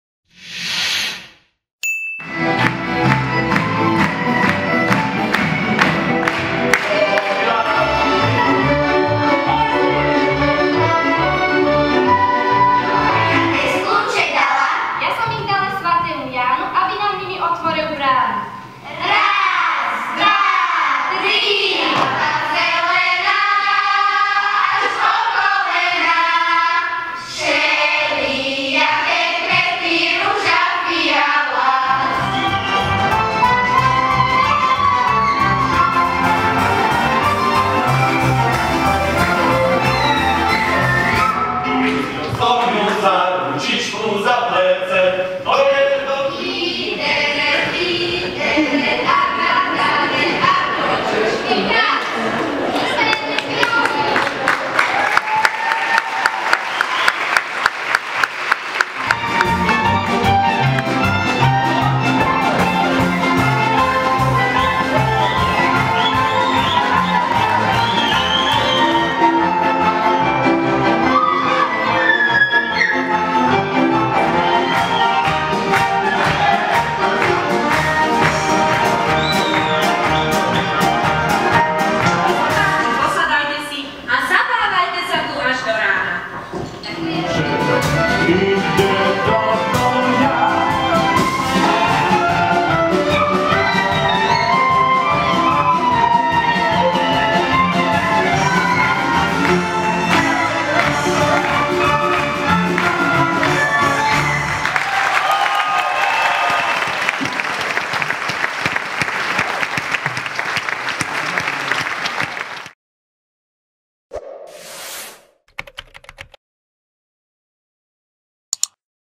Krakoviansky Lusk oslávil štvrťstoročie veľkolepo
Krakoviansky folklórny súbor Lusk
oslávil veľkolepým koncertom 19.
februára na doskách Domu umenia
Piešťany 25 rokov pôsobnosti.
Umelci divákom predviedli jedinečný
program, zachytávajúci rok v ľudovom
šate, pomohli im pritom bývalí
členovia súboru, deti z FS Krakovanček,
ľudové hudby Borovienka
a Adama Vakoša.